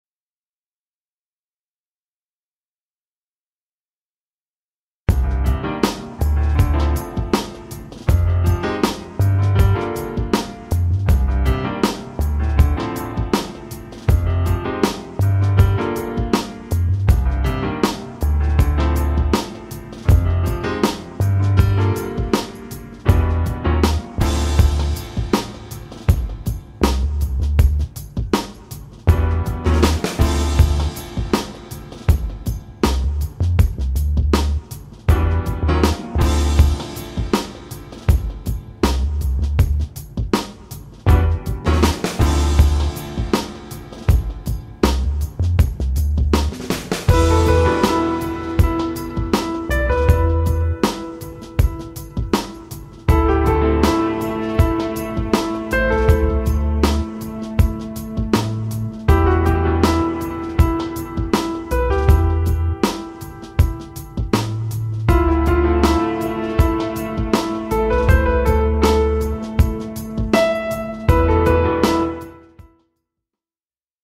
Renault clio occasion visible à Besancon présentée par Renault besancon
Cette Renault clio occasion disponible à Besancon dans le 25 est mise en vente par Renault besancon - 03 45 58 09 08 - tous les détails sont